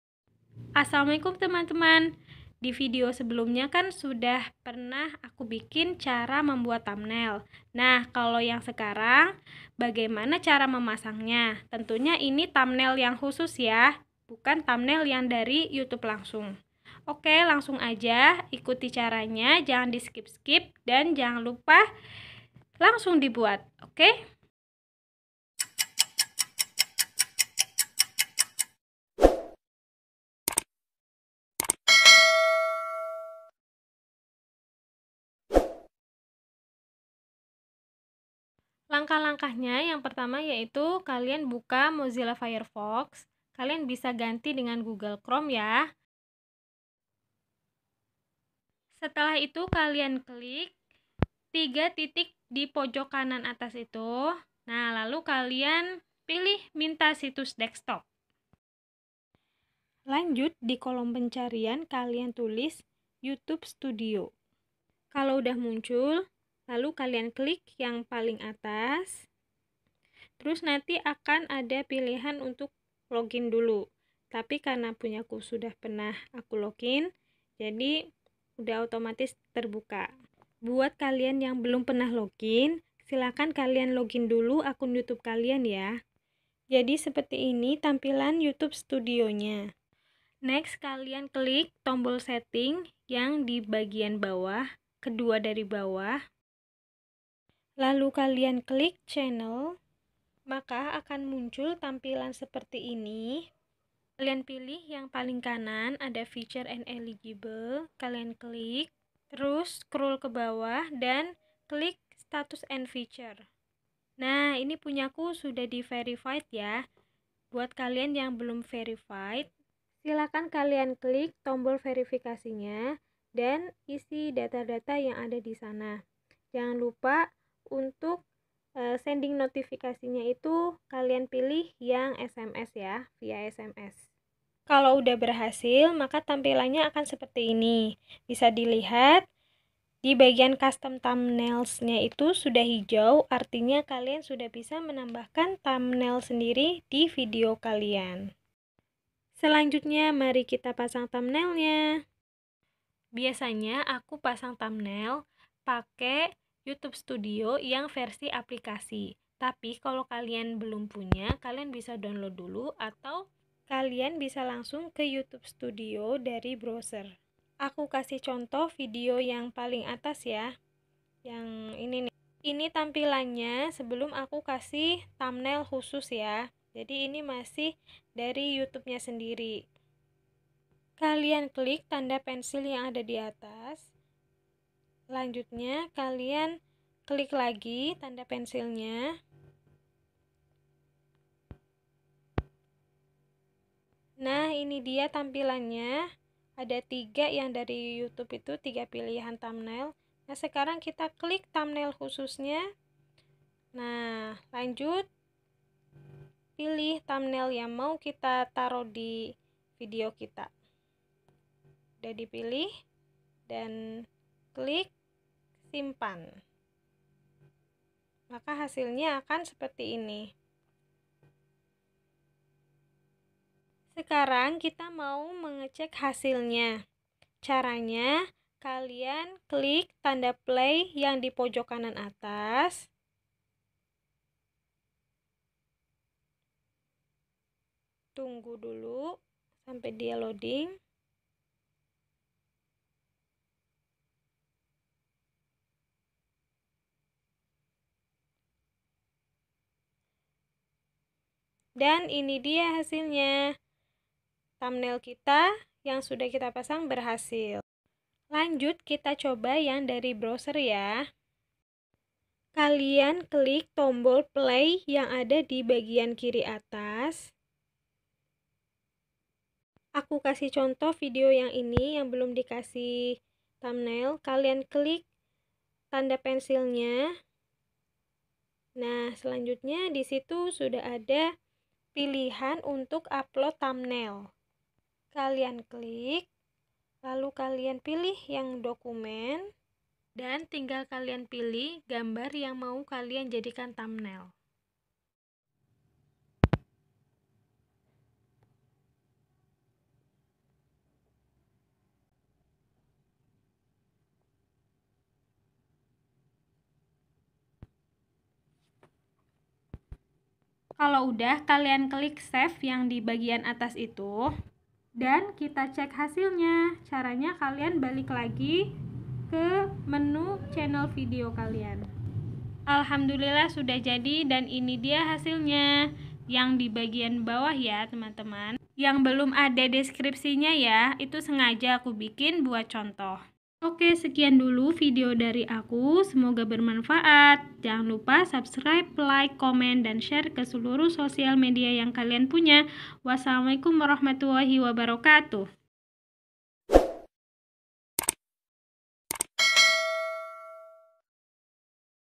Cara Memasang Thumbnail Khusus di Youtube
Assalaamualaikum wrwb teman2,,
setelah sebelumnya aku bikin video "cara membuat thumbnail youtube" sekarang adalah part 2 nya, yaitu "cara memasang thumbnail youtube" tentunya ini yang custome ya. yuk simak selengkapnya di d video. tonton sampe habis ya 🤗

Tonton video sebelumnya ya
Cara membuat thumbnail